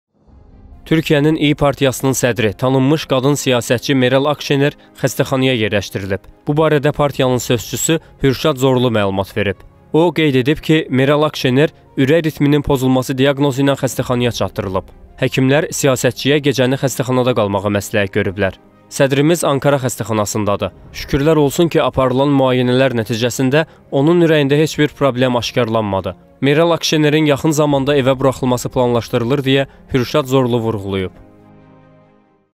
Xəstəxanaya yerləşdirilən Meral Akşenerin son vəziyyəti bilindi - BAKU TV
Türkiyənin “İyi” Partiyanın sədri, tanımış qadın siyasətçi  Meral Akşener xəstəxanaya yerləşdirilib.
Bu barədə partiyanın sözçüsü Hürşad Zorlu məlumat verib. O qeyd edib ki, Meral Akşener ürək ritminin pozulması (aritmiya) diaqnozu ilə xəstəxanaya çatdırılıb. Həkimlər siyasətçiyə gecəni xəstəxanada qalmağı məsləhət görüblər.
“Sədrimiz Ankara Xəstəxanasındadır. Şükürlər olsun ki, aparılan müayinələr nəticəsində onun ürəyində heç bir problem aşkarlanmadı. Sabah Meral Akşenerin evə buraxılması planlaşdırılır”, - deyə Hürşad Zorlu vurğulayıb.

Xəstəxanaya yerləşdirilən Meral Akşenerin son vəziyyəti bilindi